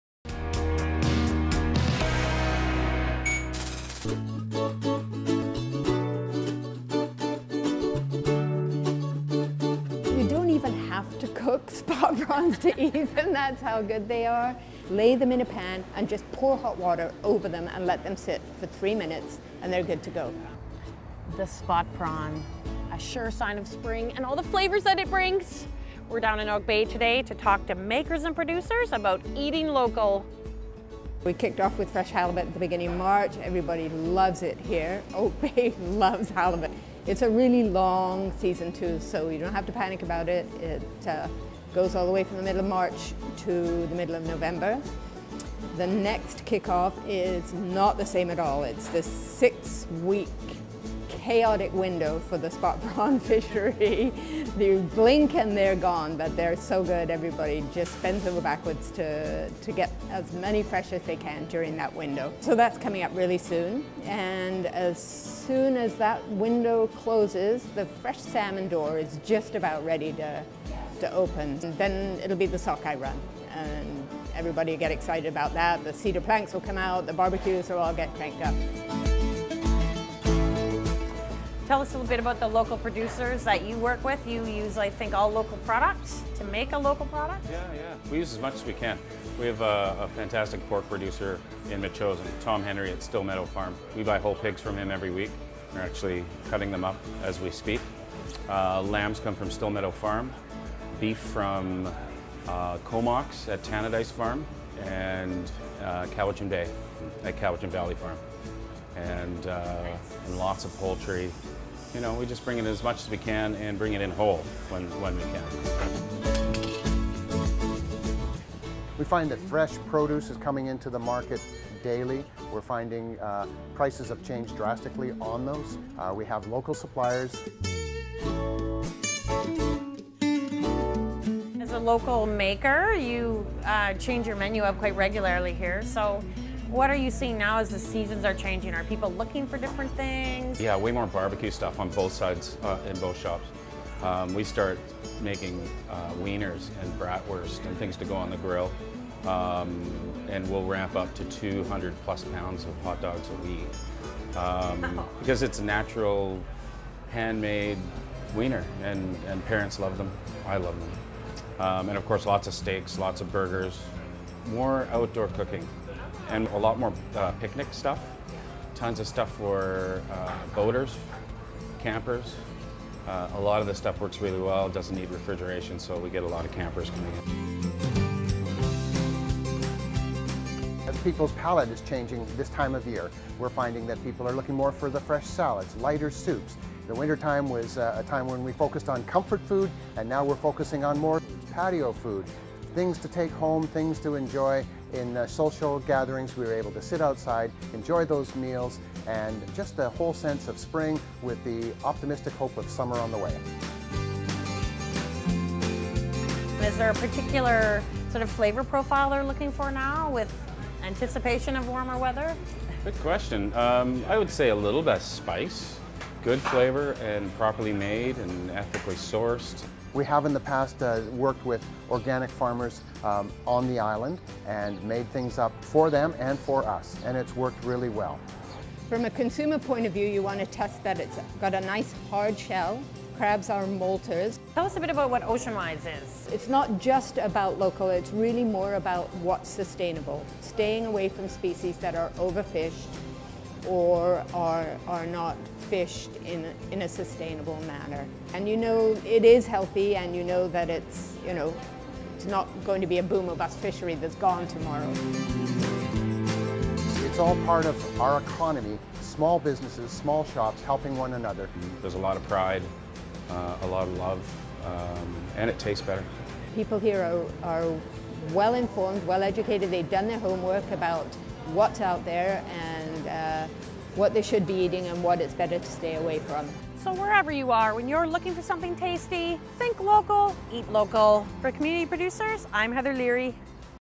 Eating Local Food
Oak Bay Village is becoming a hub for local food with a focus on in-season products. Locally sourced food with ethical & sustainable practises that are a source of pride. The Whole Beast, Oak Bay Seafood & Chef on the Run share their thoughts on spring flavours.